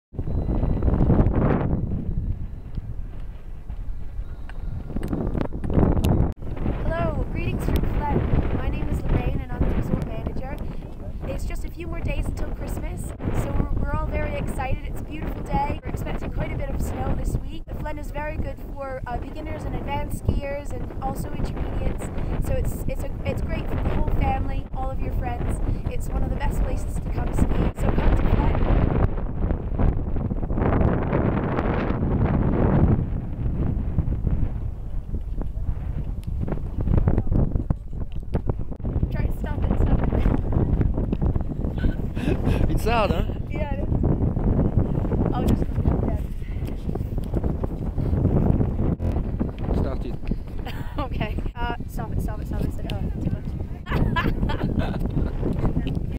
SKI Flaine, Grand Massif with Inghams
Come to Flaine, France, Skiing or Snowboarding with Inghams it is great for the whole family as Flaine and the Grand Massif ski have pistes that cater to skiers from beginners, all levels of Intermediates up to advanced.  This is my first Snow Blog of the season and we are expecting it to snow more Christmas Day!  More to come as the winter progresses.  Hope to see you on the slopes soon!
I had to ad that gag reel at the end!